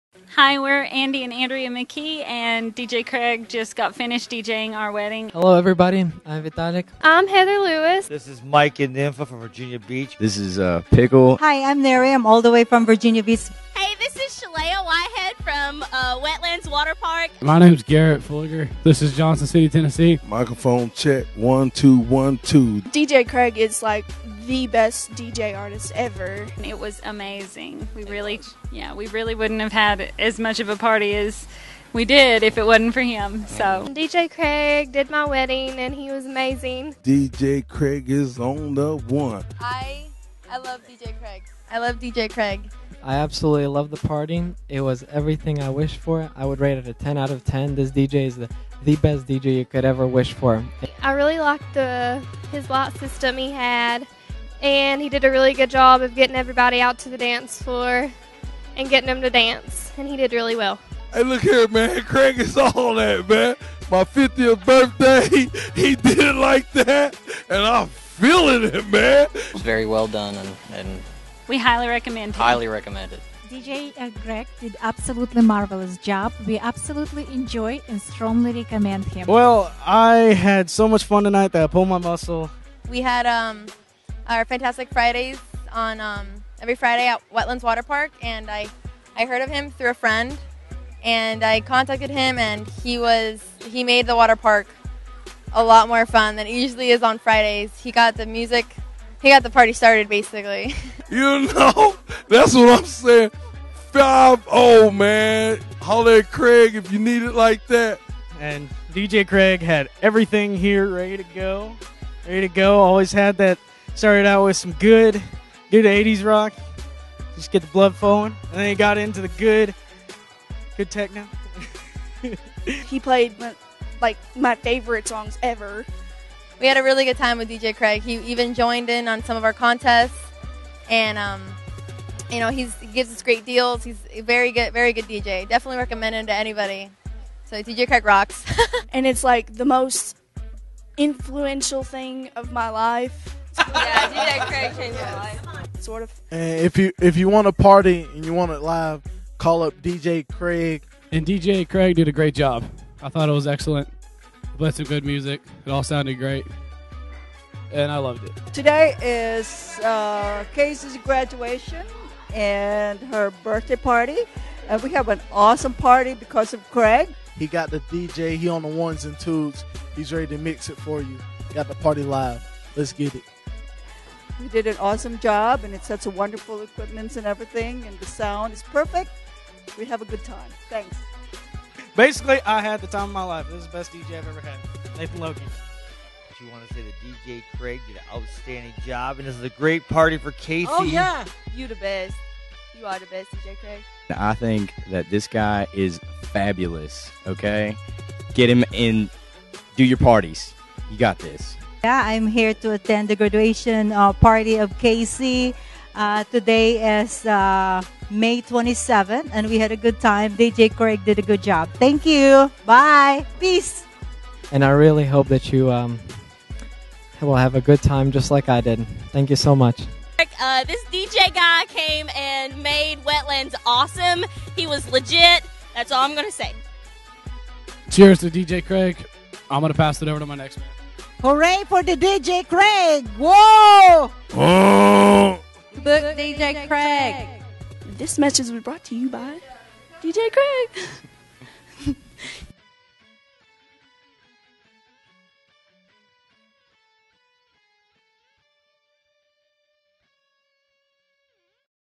DJ Craig's Video Testimonials
These are recorded video testimonials from happy clients and party guests who have experienced DJ Craig's mobile DJ services.